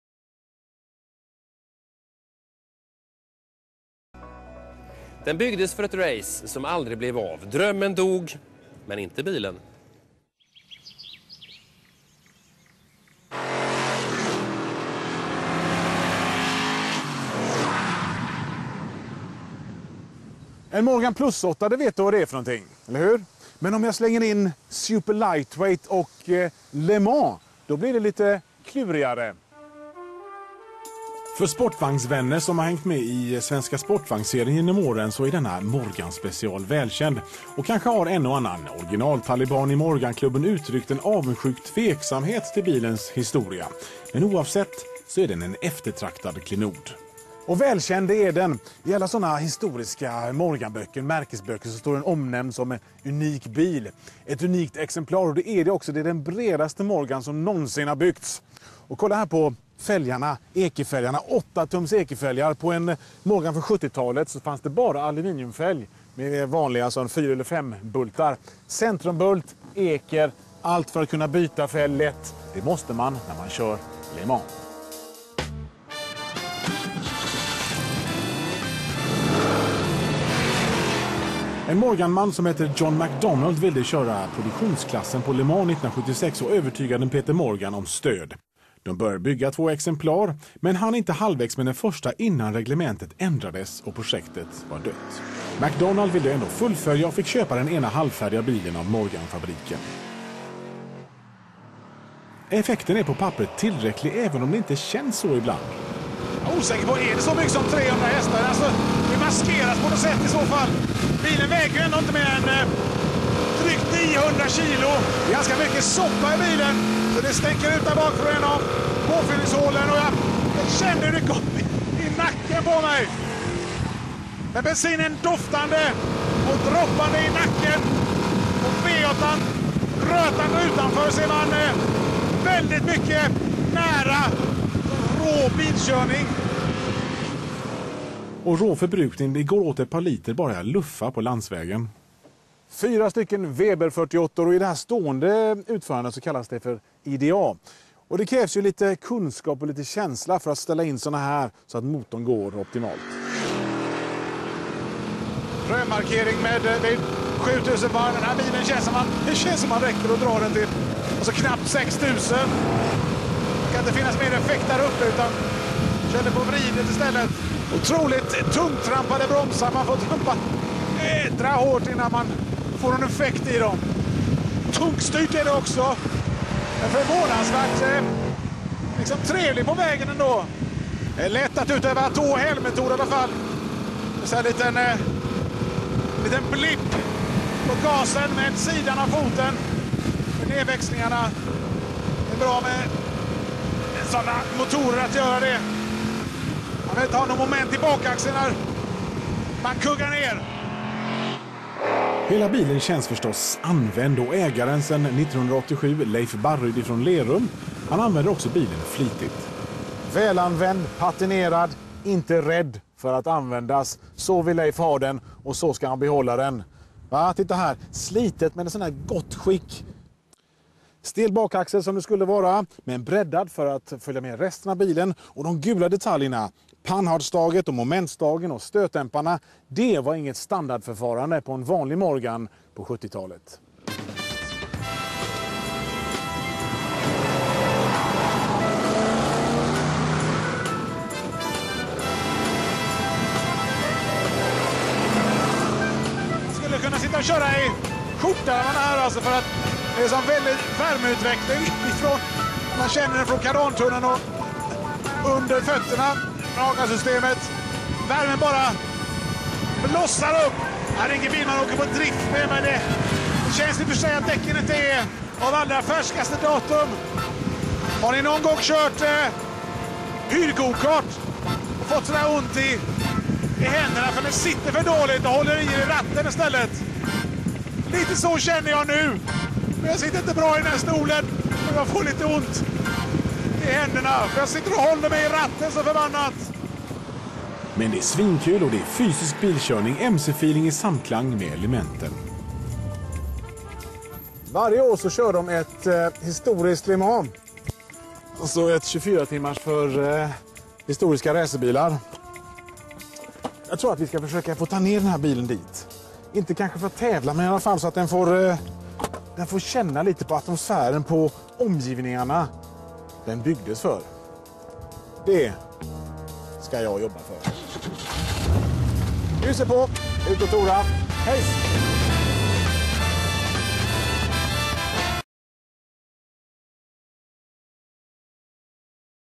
MORGAN +8 Le Mans Plus8 Car
This unique MORGAN Super Sports  Lightweight was built by the Morgan Motor Company for Le Mans 1976. The fearsome beast is powerd by a 3,5 litre Traco Oldsmobile V8 with quadruple WEBER 48 IDA carrburettors. The engine develops 350 BHP and it accelerates to more than 100 KMH in the first gear. Thi car is the widest Morgan ever built